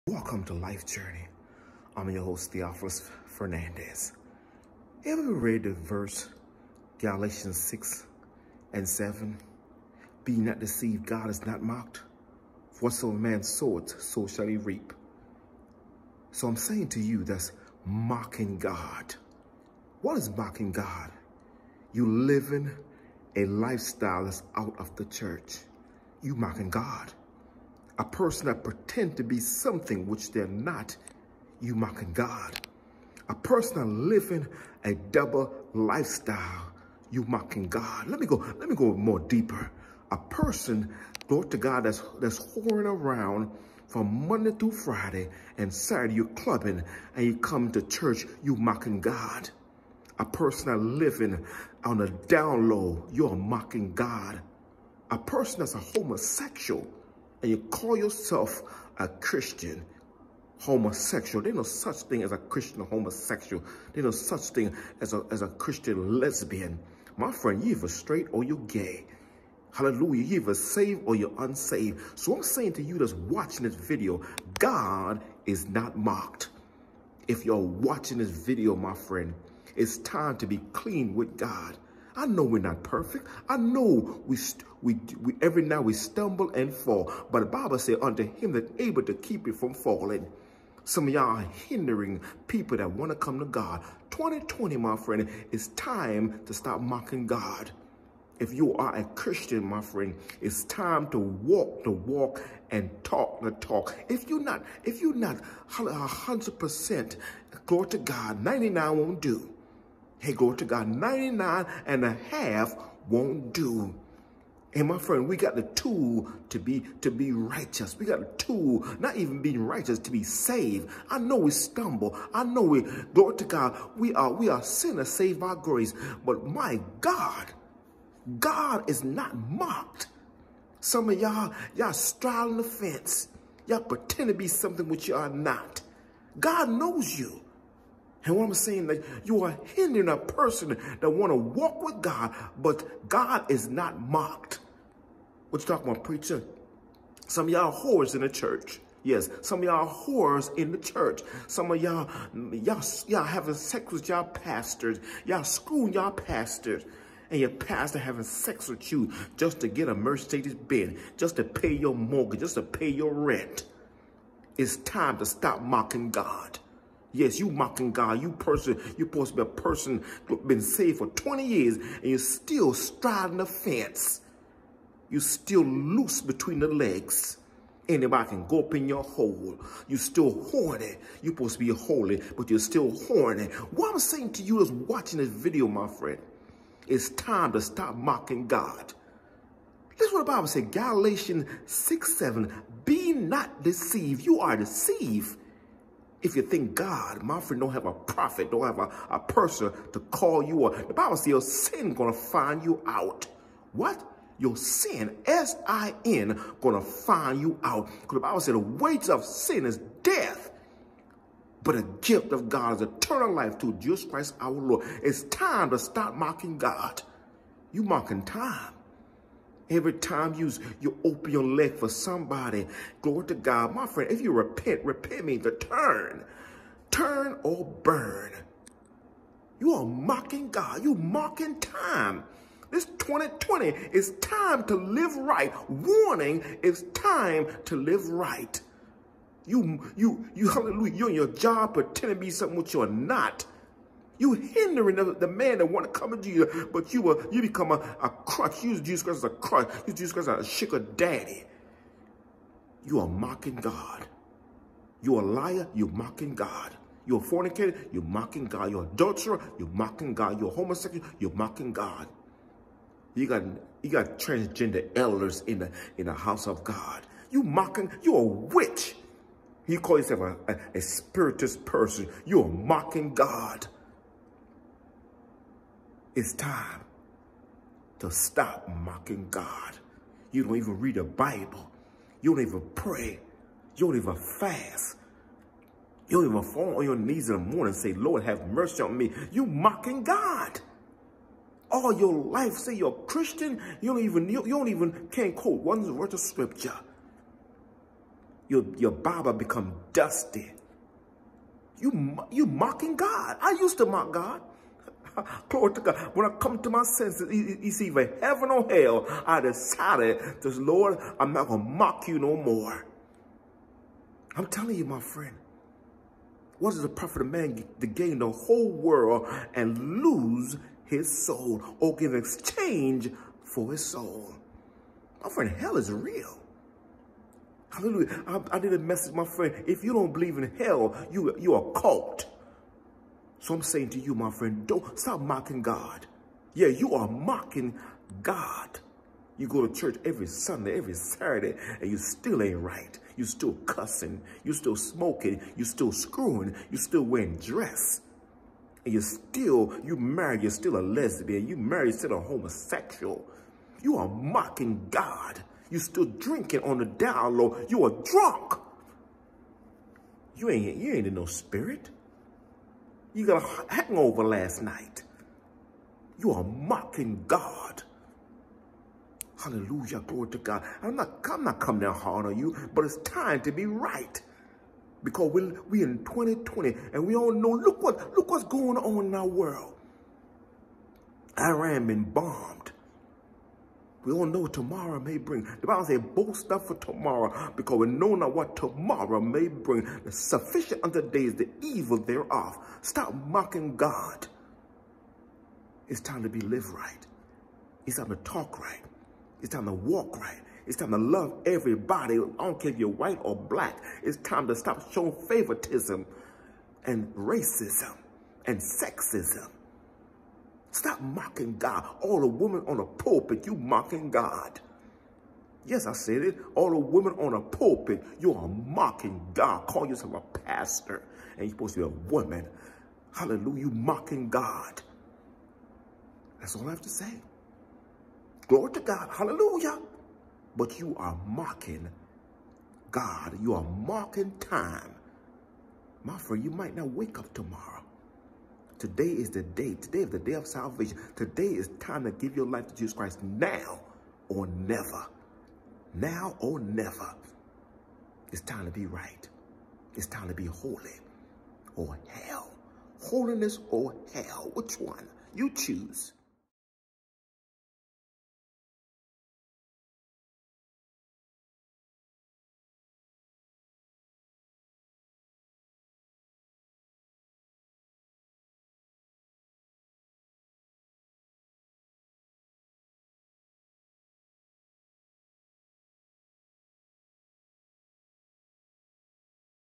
People that’s Mocking God ( Galatians 6:7 )
Those who are living on the fence or living a triple lifestyle you are mocking the living God.

Share this Channel with a million Of  People and you will save a Life Today